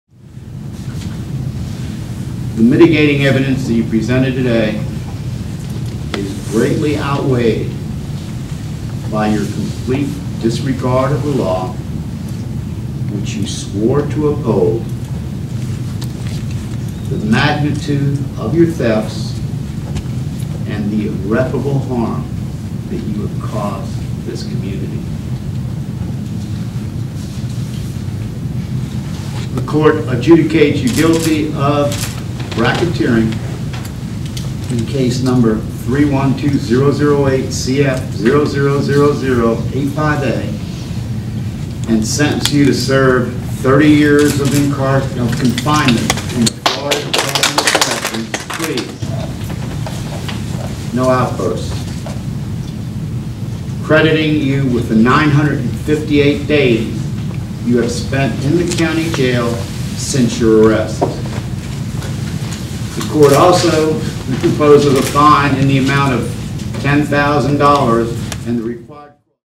Ira Hatch gets 30 years prison
VERO BEACH -- Ira C. Hatch has been sentenced to 30 years in prison - the maximum the court could have handed down. He will receive credit for 978 days he has already spent in the county jail awaiting the outcome of his trial. Staff video by Keith Carson.